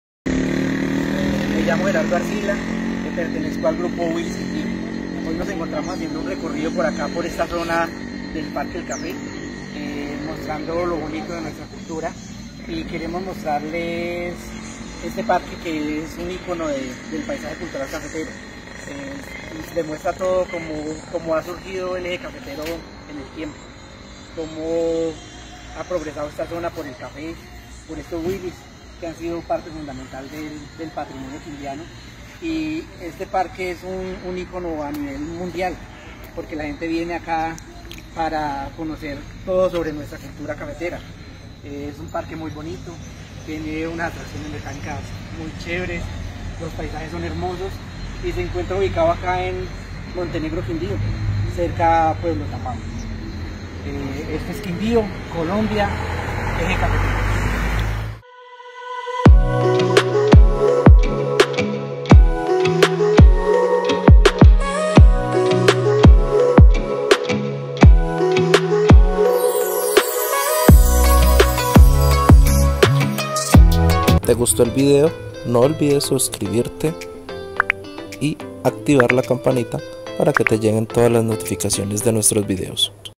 En el monumento del parque del café
esta es la torre mirador ubicada a la entrada del parque del café